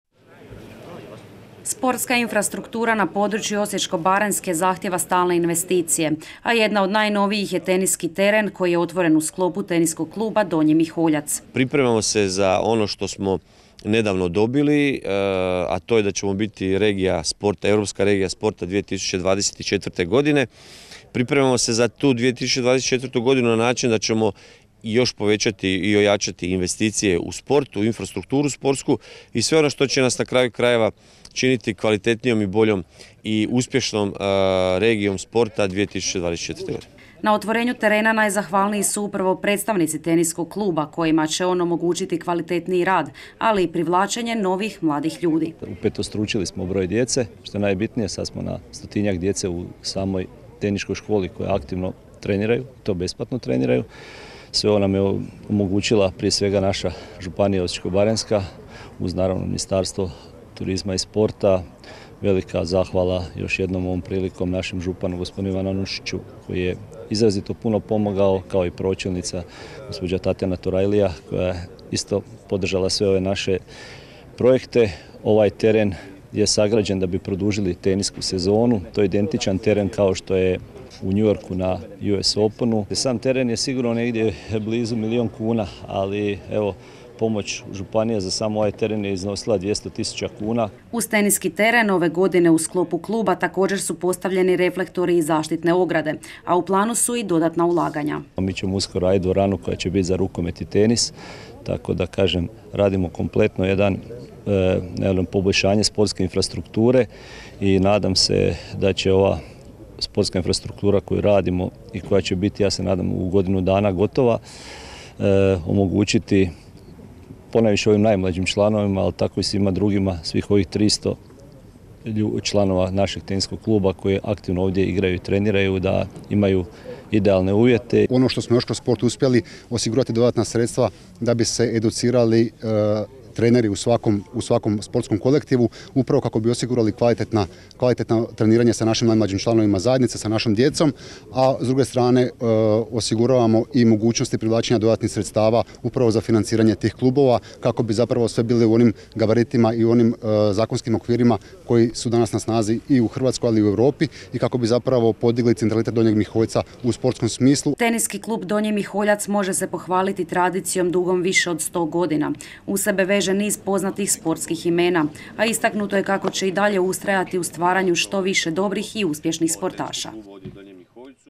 Donji Miholjac dobio novi teniski teren vrijedan više od milijun kuna
Stanovnici Donjeg Miholjca dobili su novo mjesto za sport i rekreaciju. Uspješno su završeni radovi na novom teniskom terenu koji je osvanuo u sklopu Teniskog kluba Donji Miholjac, a vrijednost investicije iznosi više od milijun kuna.